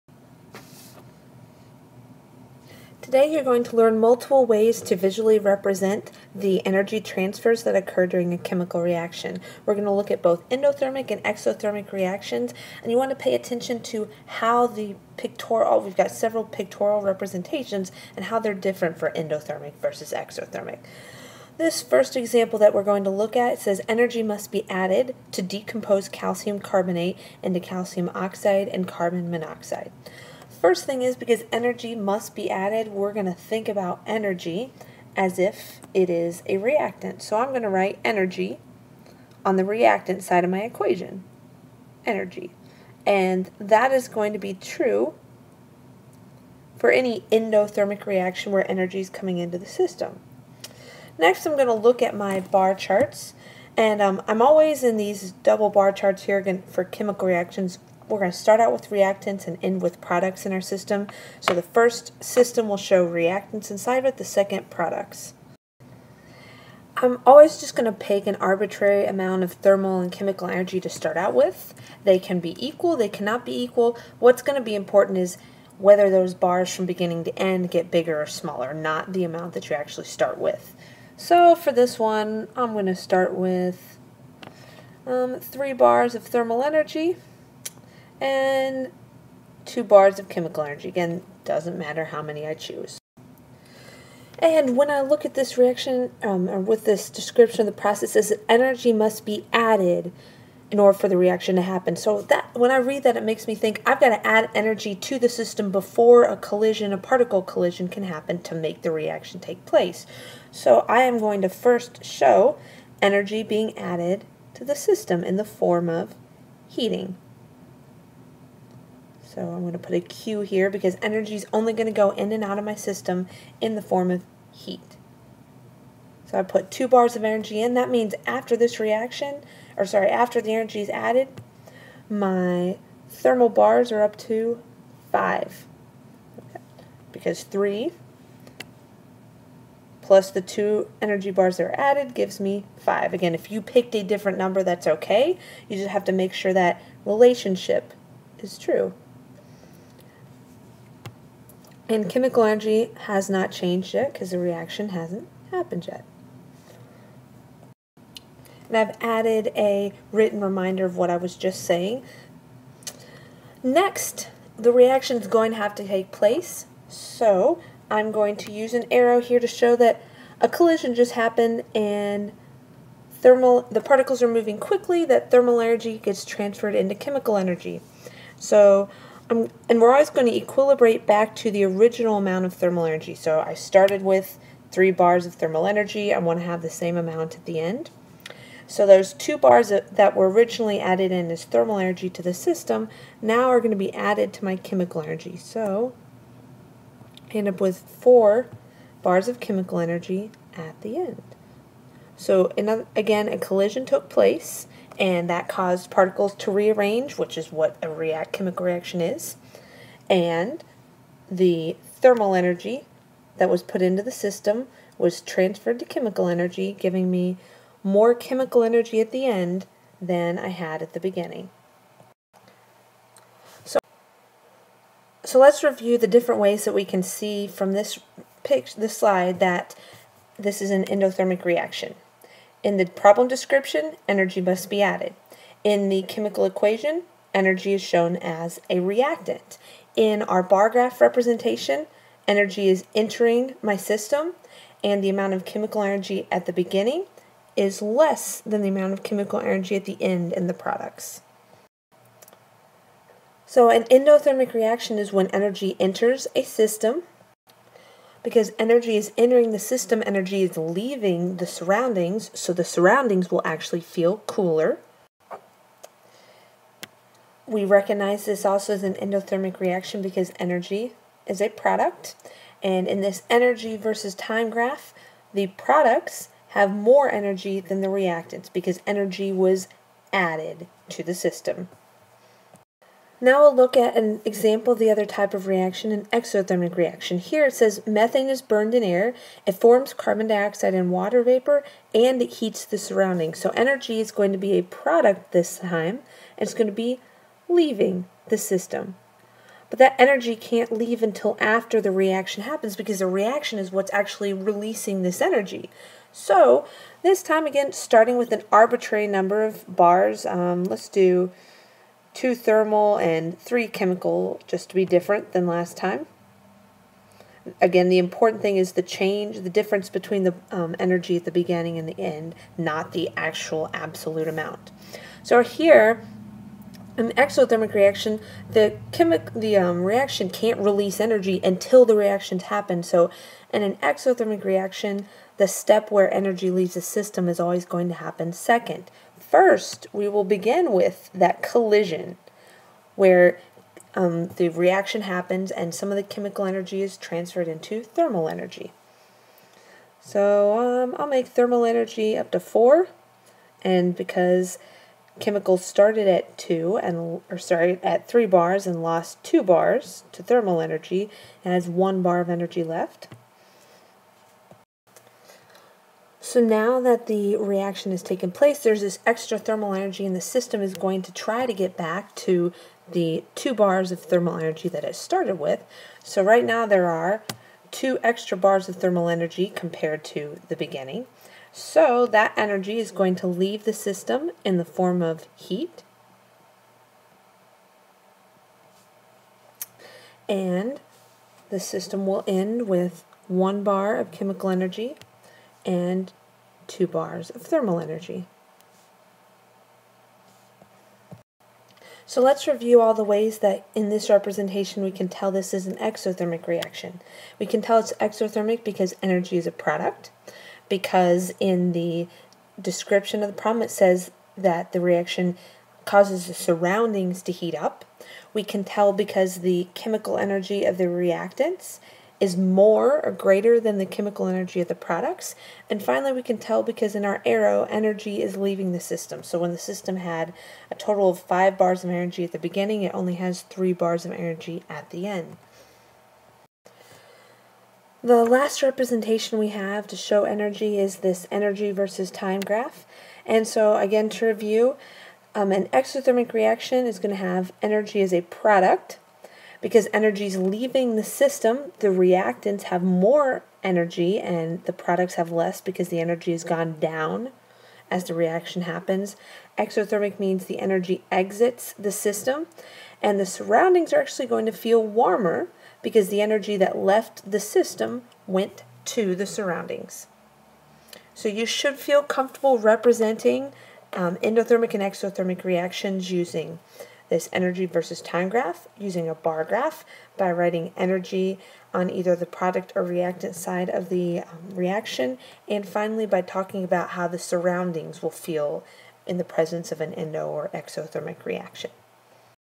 Chem Unit 7: Chemical Energy Bar Charts
How to create energy bar charts for endothermic and exothermic reactions.